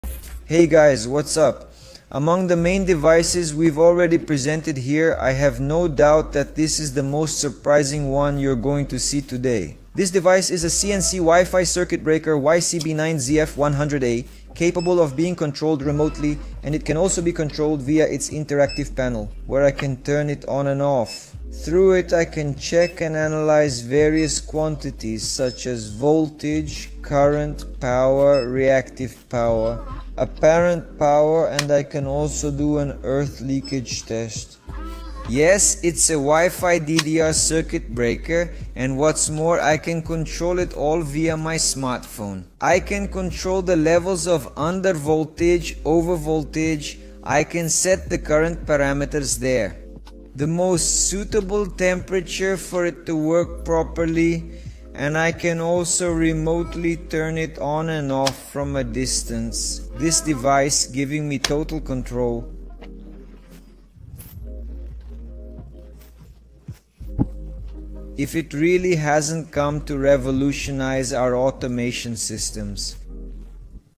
The CNC Electric Wi-Fi Circuit Breaker YCB9ZF-100AP— all in one smart device.
This  combines leakage protection, C100 overload safety, and remote monitoring. It's a reliable solution for smart homes and building automation, with Wi-Fi control and real-time power data.
Follow us for more intelligent solutions in electrical protection and automation.